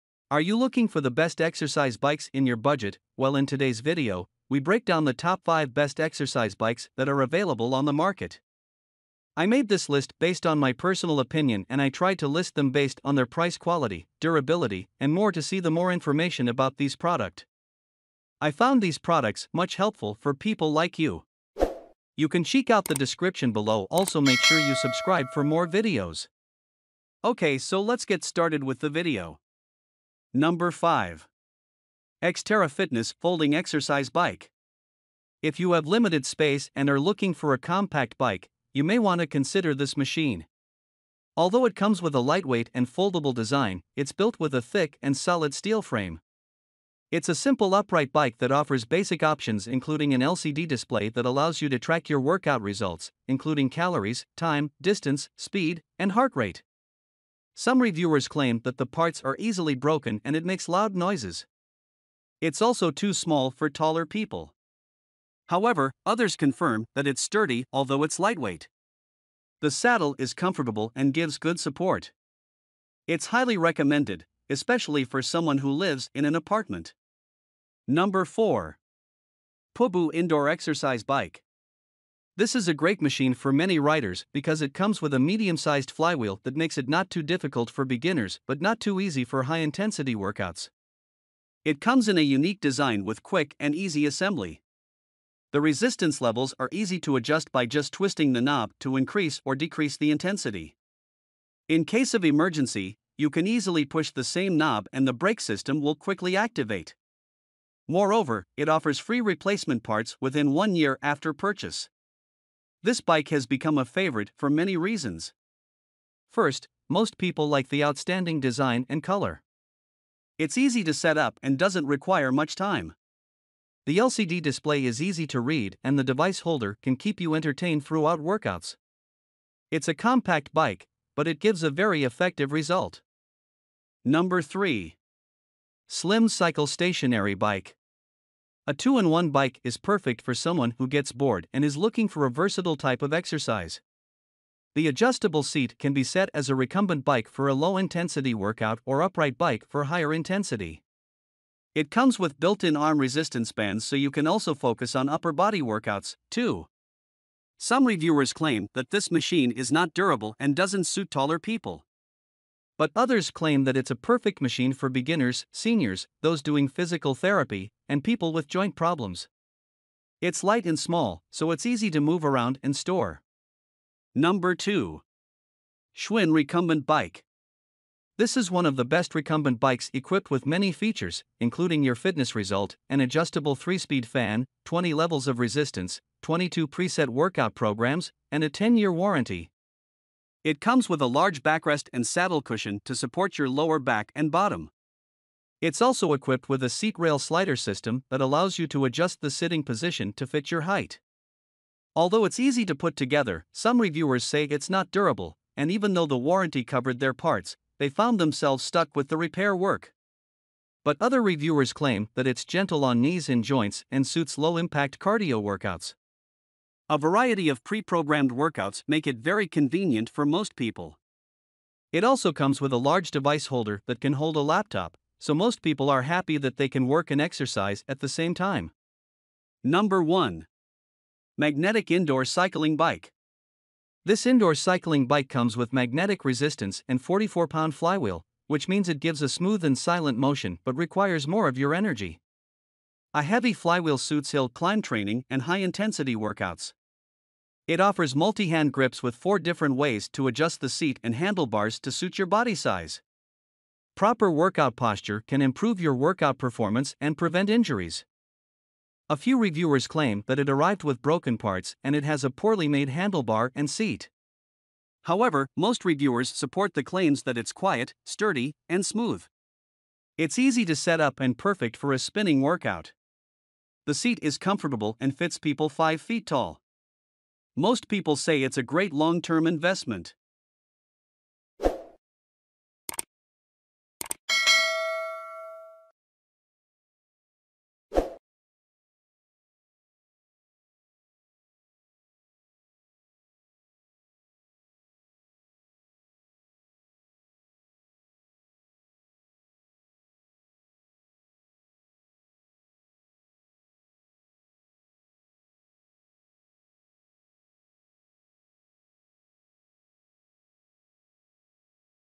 Top 5 Exercise Bikes - Best Exercise Bikes
In this video we are short listed the Top 5 Exercise Bikes - Best Exercise Bikes on the market. We made this list based on our personal opinion.

5.  XTerra Fitness Folding Exercise Bike

4.  Pooboo Indoor Exercise Bike

3.  Slim Cycle Stationary Bike

2.  Schwinn Recumbent Bike

1.  Magnetic Indoor Cycling Bike